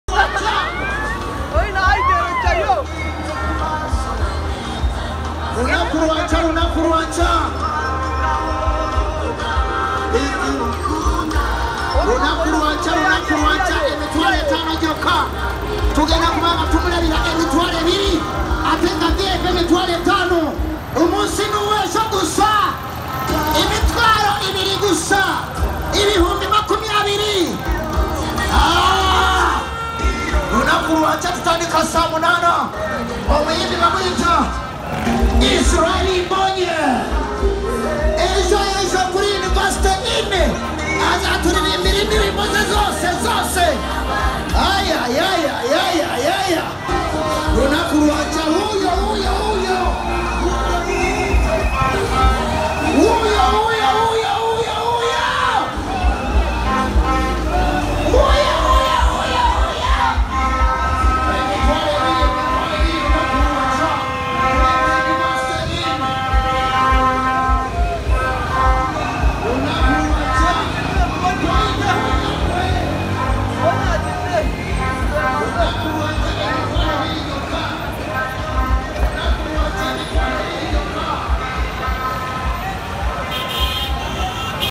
ISRAEL MBONYI IN UGANDA MBARARA CITY
RWANDAN ARTIST ISRAEL MBONYI LIVE IN UGANDA MBARARA CITY#film